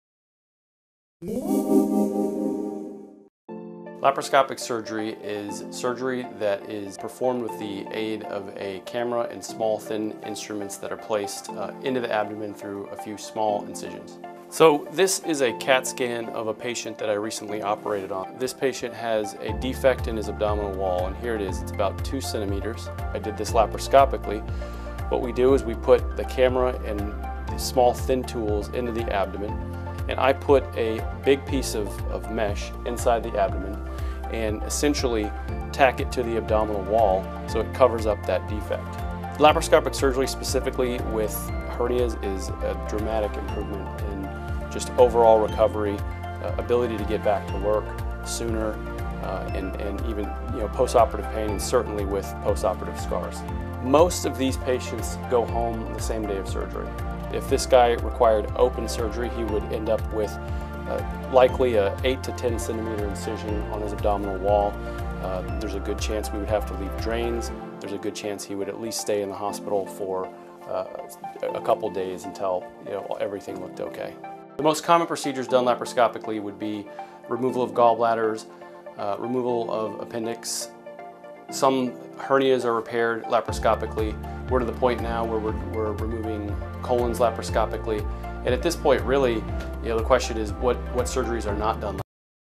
Understanding Laparoscopic Surgery
Dr. Giacomo Meeker, surgeon at The Surgical Clinic, eplains the benefits of minimally invasive sugery : the Laparoscopic Approach.
TheSurgicalClinic.com/general-surgery/